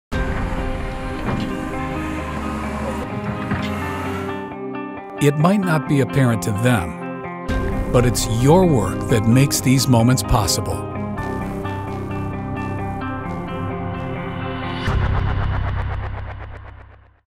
Snow Plow Operators Clear Way for School Bus - Pro-Tech Sno Pusher
Sno Fighters are ready for chaos at a moments notice.  And they push through adversity like they push snow.

Thank you to all of those pushing and plowing snow and keeping their communities operating during the winter months.